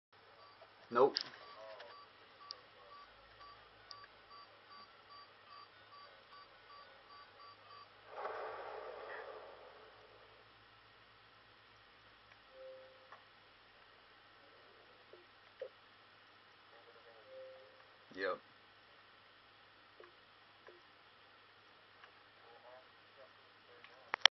Map 3 vs. 2 Nutz and their Boltz
We lost first map. Second map we lost 4-3 but they hosted with wrong rules (no killcam), and 3rd map we won 4-2.